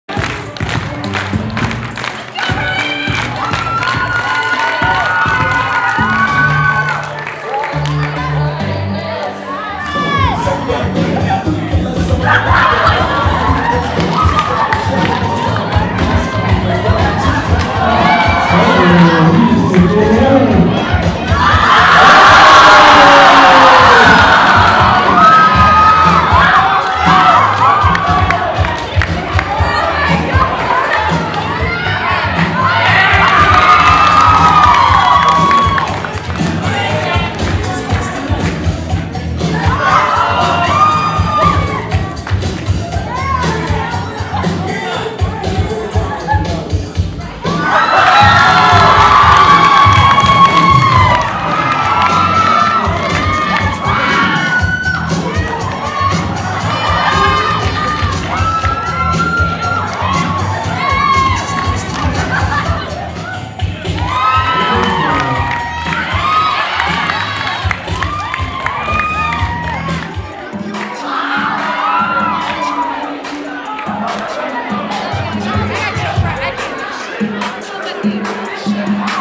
Video: Hanover-Horton Students dance off at prom
Hanover-Horton students have a dance off during their prom.